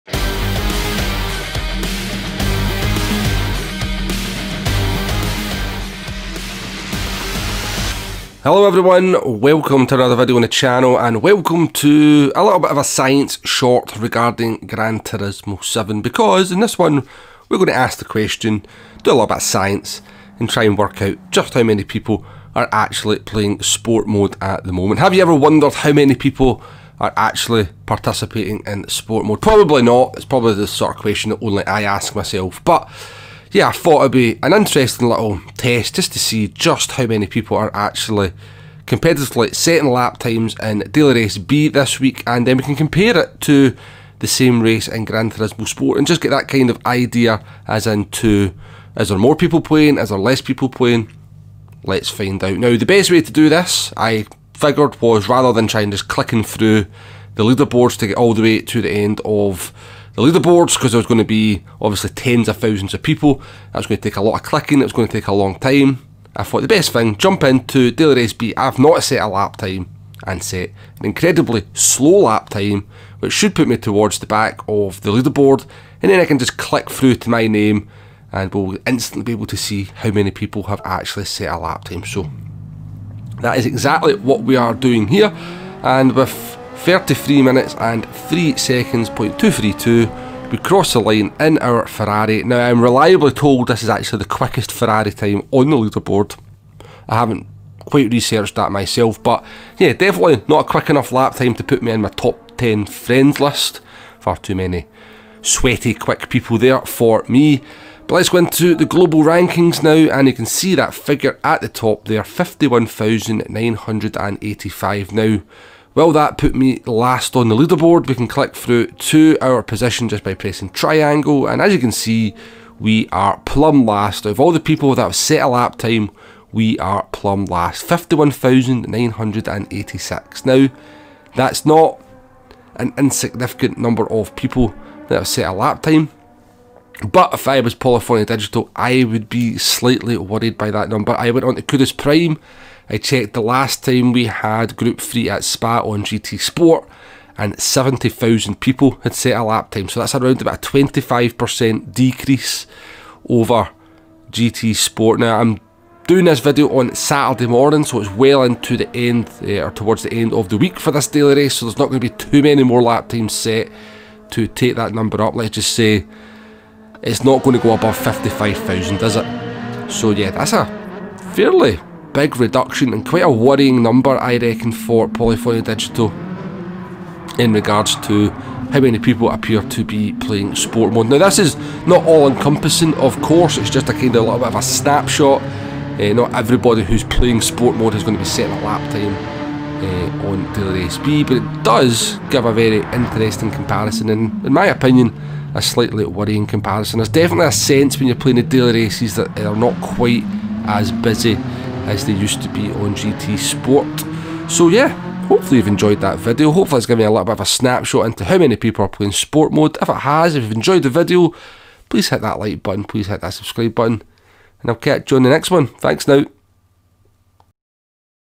Gran Turismo 7 science...How many people play sport mode??
Ever wondered how many people are playing sport mode in Gran Turismo 7?. Lets use some science to find out